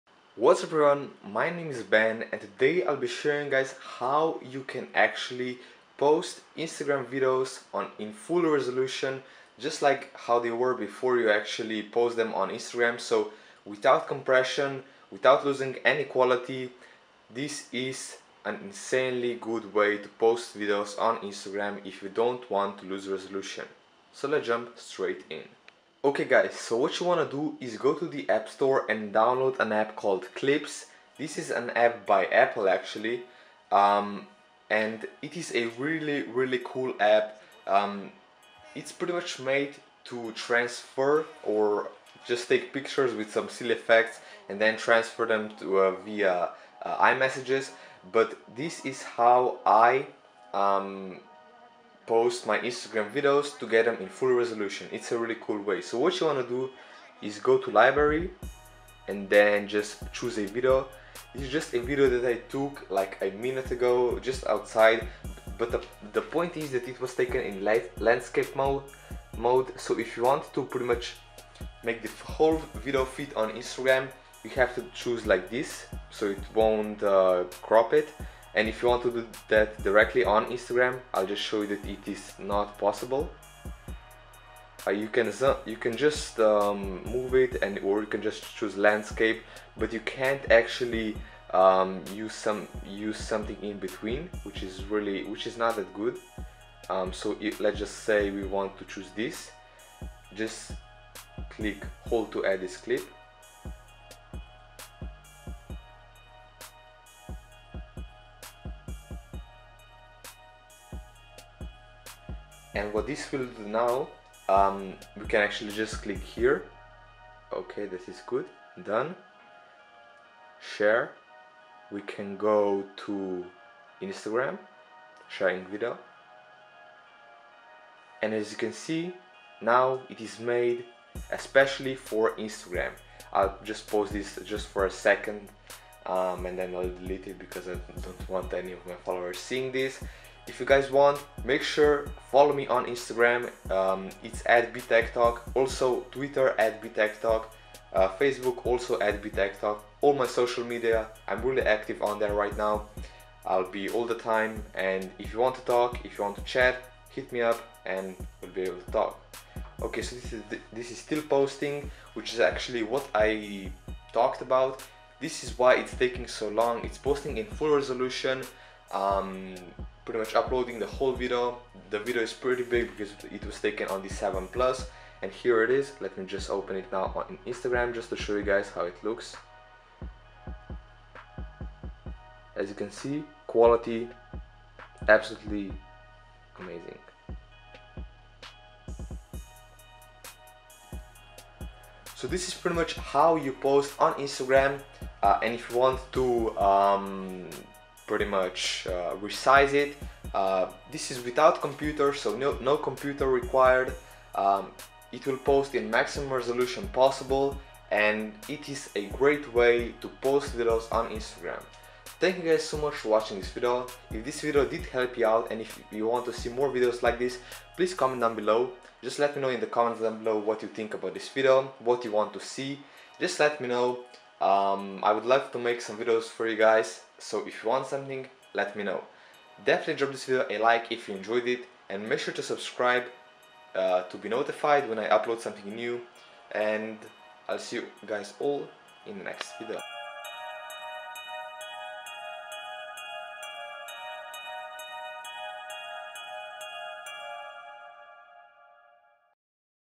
How I Post Videos on Instagram in FULL Resolution! (No Editing Required!)
This is a tutorial about posting videos on Instagram in full resolution, this works on any iOS device without any editing, no computer is required.